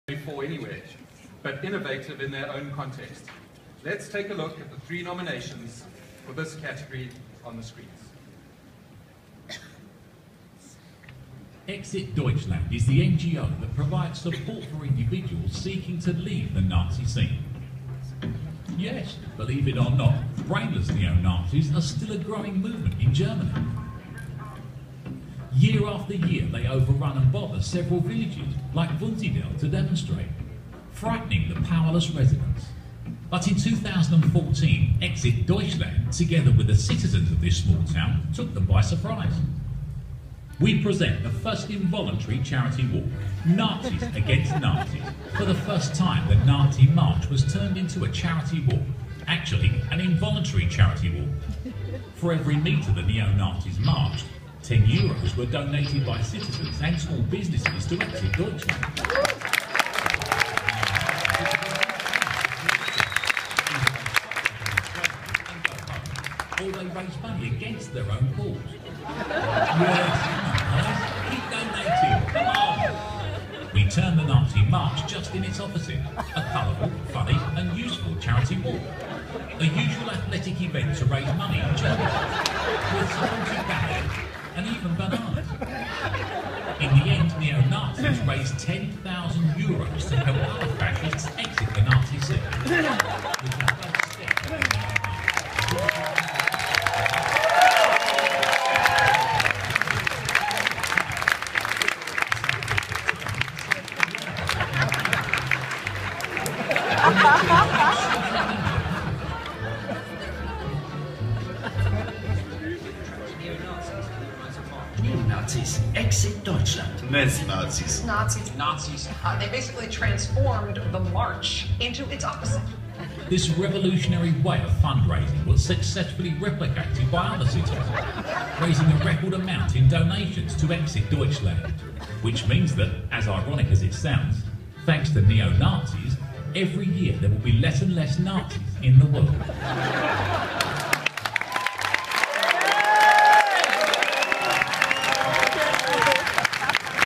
FUNDCAST #117 - Exit Deutschland - NAZIS against NAZIS - Global Awards for Fundraising 2015
The campaign "Nazis against Nazis" (first involuntary walkathon in history!!) was nominated for the "Global Awards for Fundraising" by The Ressource Alliance at the International Fundraising Congress 2015. Category: Innovative Fundraising Campaign. And the winner is. (see next video!)